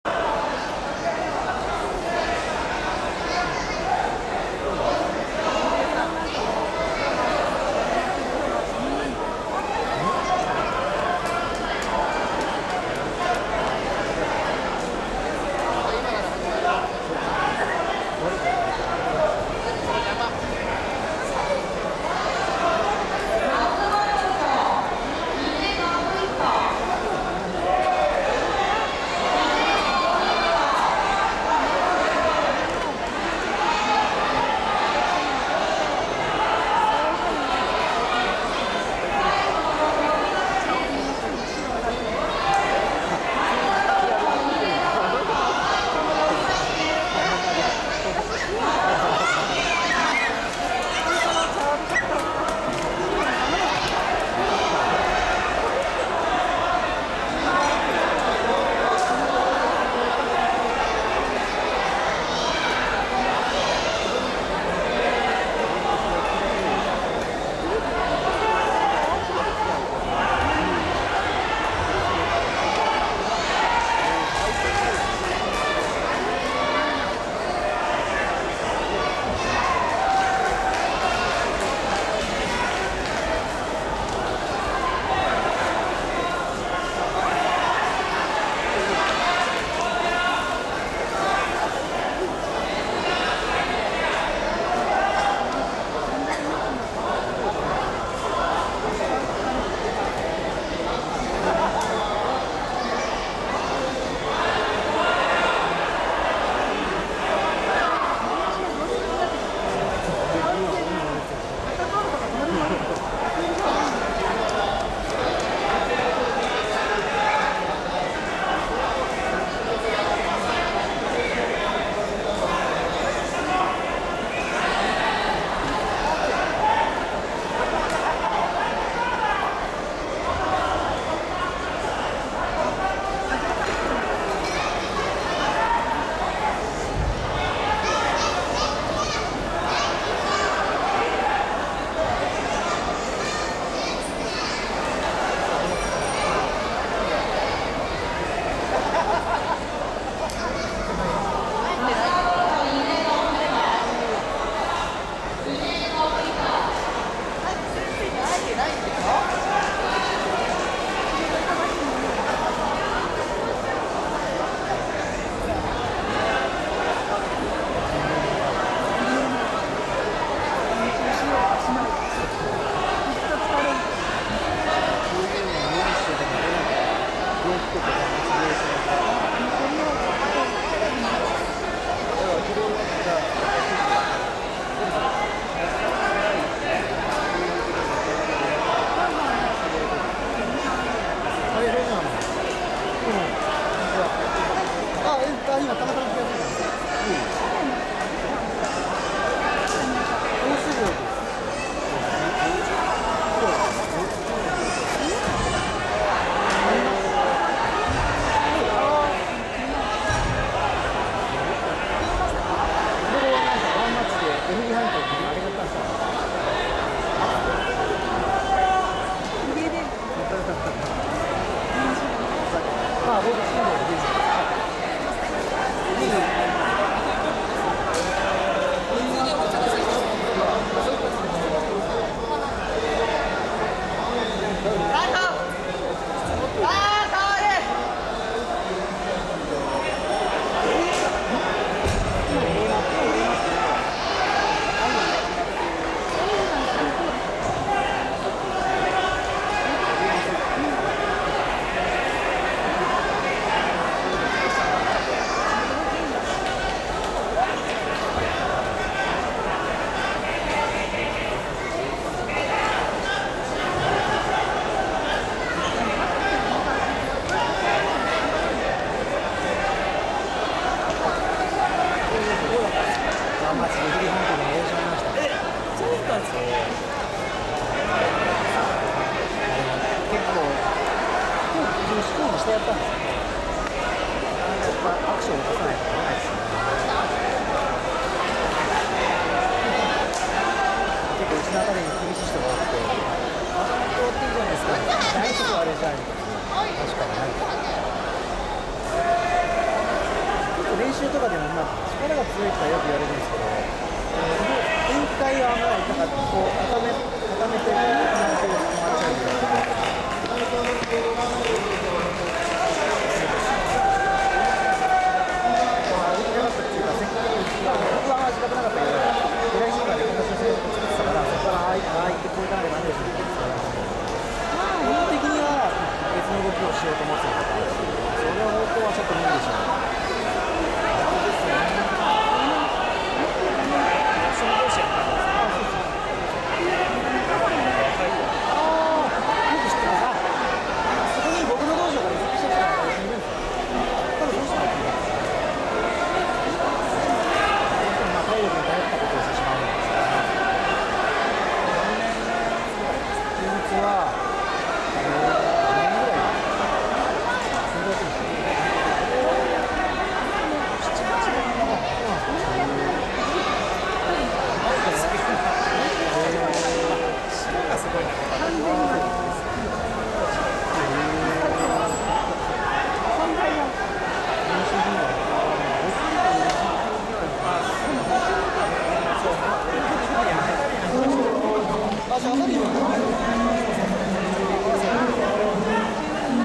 Nakamura Hiroshi vs Opponent
コパブルテリア
2013 Copa Bull Terrier BJJ tournament in Tokyo Japan